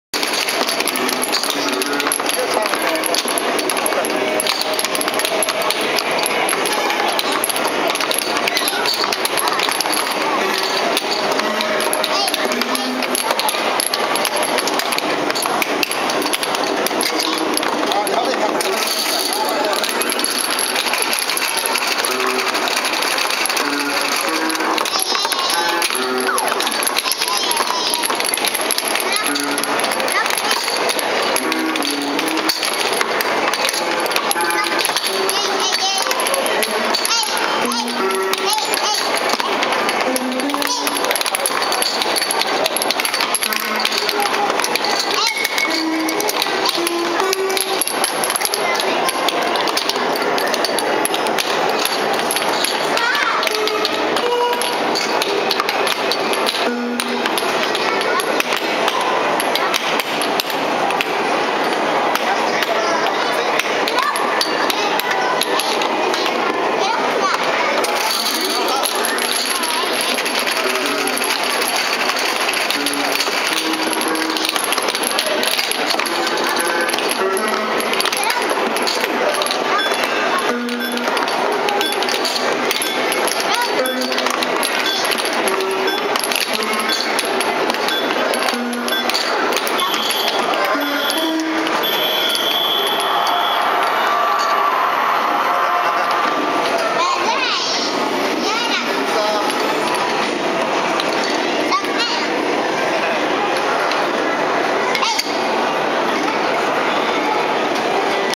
ビッグワンスマッシュ 初プレイ
ミニパックが大量に(~o~)
おもしろそうなので初めてプレイしてみました!(^^)!
ビッグワンが入れば１００点、ミニは３０点。

２才チビは、音にビビって離れて、嫁撮影で、
チハル４０才（左側）VSコウキ６才（右側）
僅差で私がなんとか勝ちました(^_-)